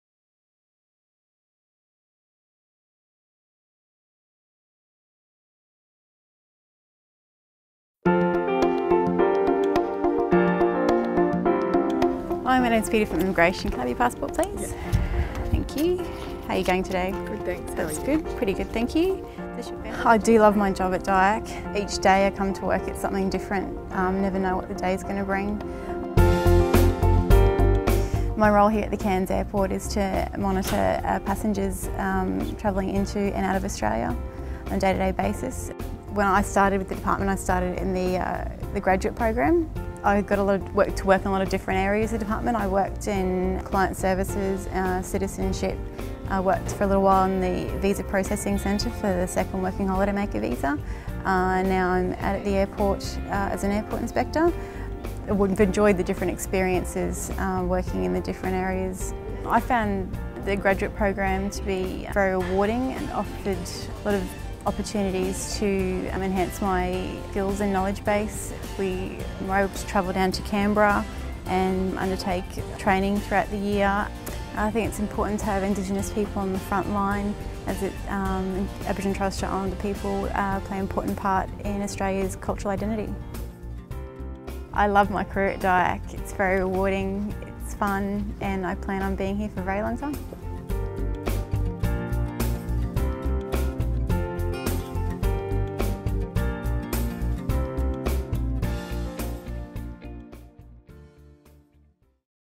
Peta's Indigenous career journey
Peta commenced in the department as an Indigenous graduate. After completing her graduate year, she took up the opportunity to become an airport inspector at Cairns airport.